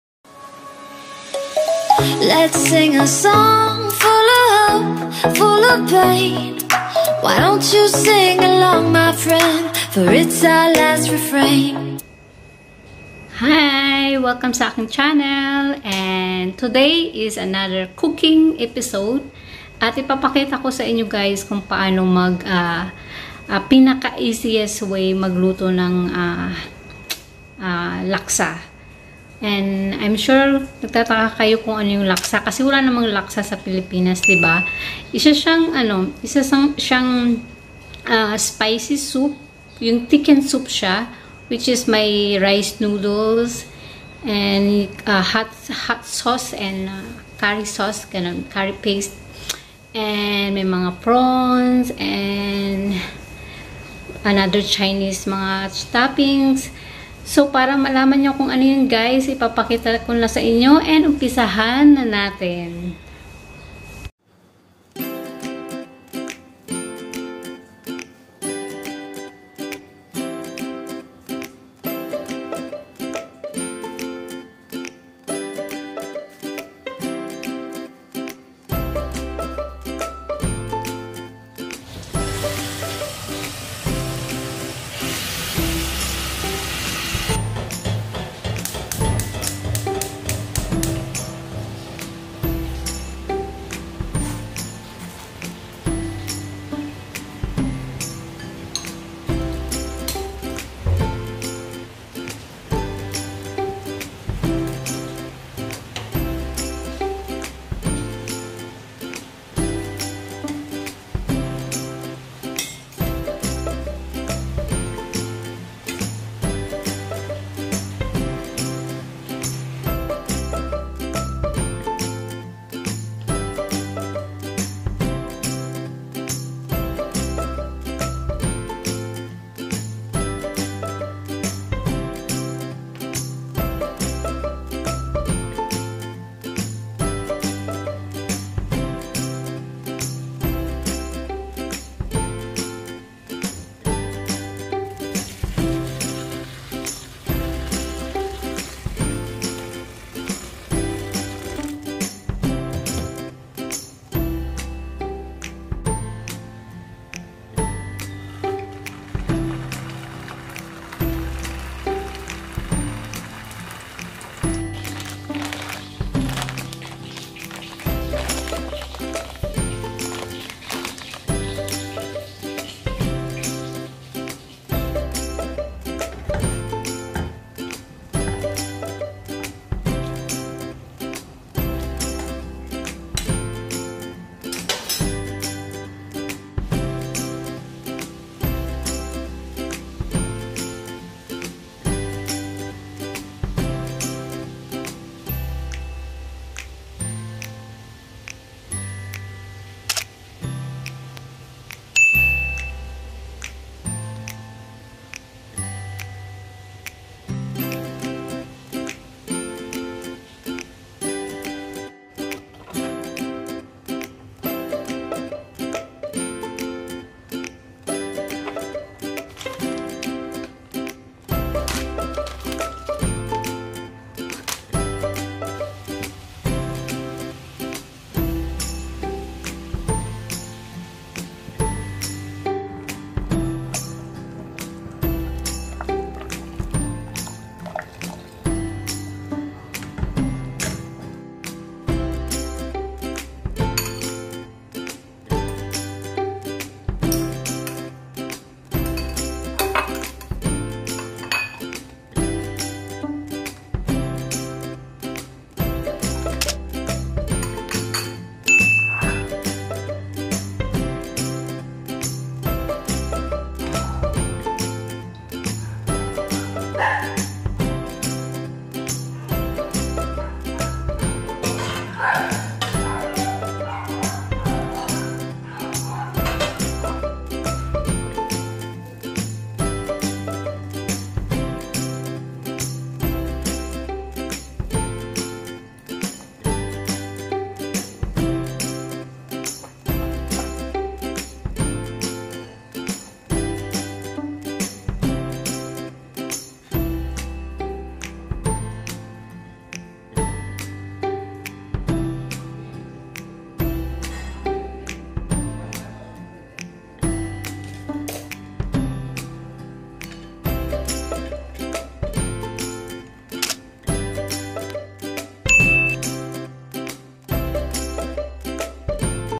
HOW TO COOK THE EASIEST WAY OF COOKING LAKSA,SINGAPORE LAKSA (shortcut)
Hello Everyone today's video is cooking episode.And i will be cooking easiest way of cooking Laksa,every Singaporeqns loves Laksa.Hope may natutunan po kayo sa ating video today..Stay safe Everyone!!

My Intro!

Genre : Pop
Mood : Happy